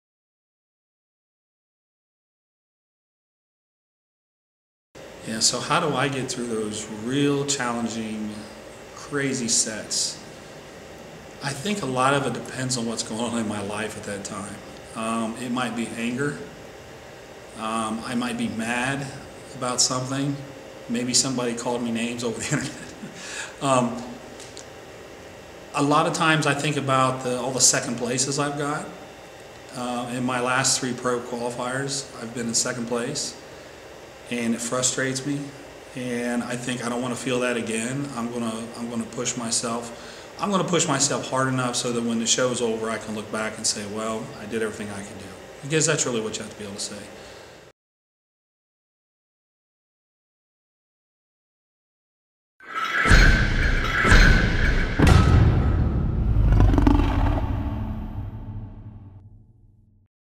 John Meadows on lifting with heart
In this IronLife issue #7 preview, top coach John Meadows gives an insight into how he gets motivated for mentally and physically challenging training sessions. You can get the full video, plus exclusive features from the world's best training and nutrition experts, by downloading the magazine via iTunes or Google Play.